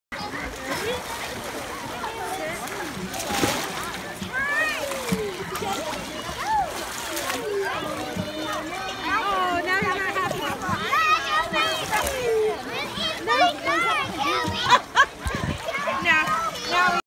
Easton and Great Auntie Valerie
At the Daybreak So Jo Pool 2019
Easton James and Valerie Siddoway